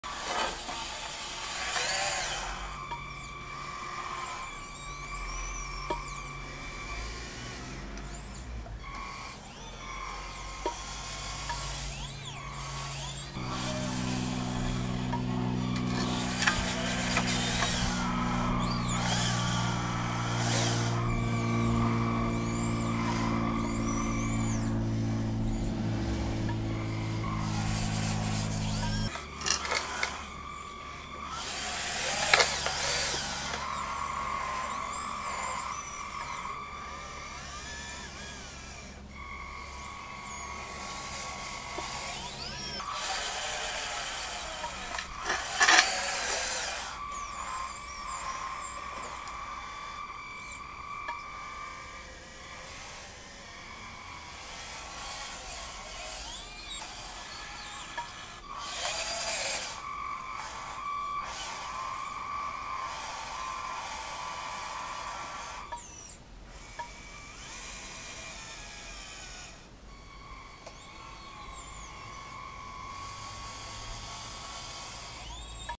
CTI / Bruder Radlader bei ertsen Baggern
Erste Probefahrt mit meinem Radlader, CTI Bausatz, versorgt über 4s LiFePo A123